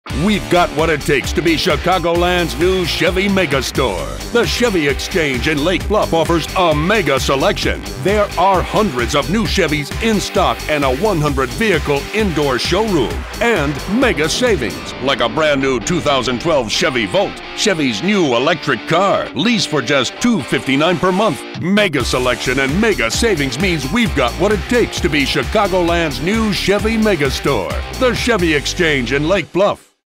TCE 7 3 12 Mega Volt
The following spot was created for The Chevy Exchange located in Lake Bluff.  This spot was pitched in several incarnations.  This version was the final version the client approved and aired.

All work was done in After Effects, Final Cut Pro, Photoshop and Particle Illusion.  Turn around time was about a day.